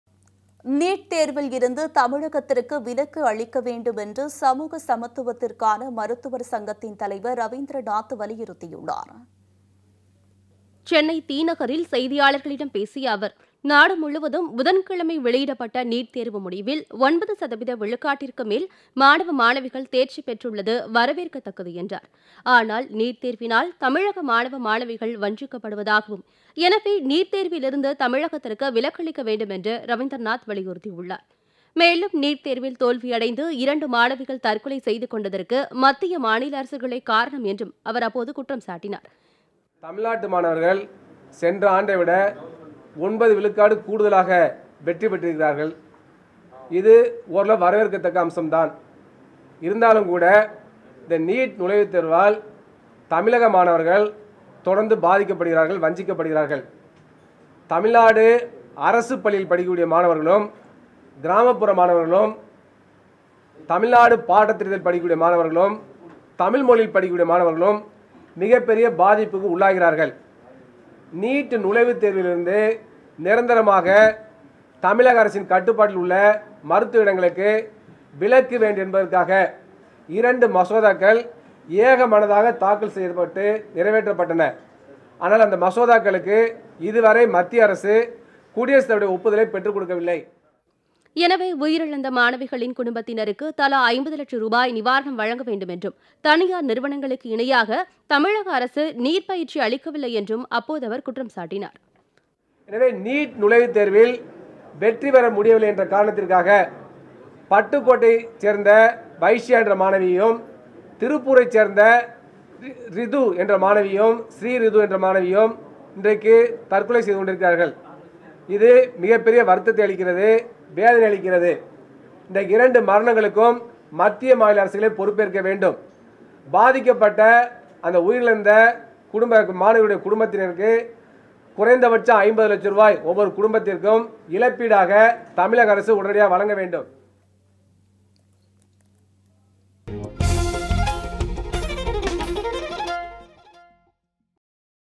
நீட்தேர்வில் இருந்து தமிழகத்திற்கு விலக்கு அளிக்க - ரவீந்திரநாத் வலியுறுத்தல்
Click here to watch Live updates on election results சென்னை திநகரில் செய்தியாளர்களிடம் பேசிய அவர், நாடு முழுவதும் புதன்கிழமை வெளியிடப்பட்ட நீட் தேர்வு முடிவில் 9 சதவீத விழுக்காட்டிற்கு மேல் மாணவ, மாணவிகள் தேர்ச்சி பெற்றுள்ளது வரவேற்கதக்கது என்றார்.  ஆனால் நீட் தேர்வினால் தமிழக மாணவ, மாணவிகள் வஞ்சிக்கப்படுவதாகவும், எனவே, நீட்தேர்வில் இருந்து தமிழகத்திற்கு விலக்கு அளிக்க வேண்டும் என்றும் ரவீந்திரநாத் வலியுறுத்தியுள்ளார்.  மேலும் நீட் தேர்வில் தோல்வி அடைந்து இரண்டு மாணவிகள் தற்கொலை செய்து கொண்டதற்கு மத்திய மாநில அரசுகளே காரணம் என்று அவர் குற்றம்சாட்டினார். எனவே, உயிரிழந்த மாணவிகளின் குடும்பத்திற்கு தலா 50 லட்சம் ரூபாய் நிவாரணம் வழங்க வேண்டும் என்றும் தனியார் நிறுவனங்களுக்கு இணையாக தமிழக அரசு நீட் பயிற்சி அளிக்கவில்லை என்றும் அவர் குற்றம்சாட்டினார்.

Byte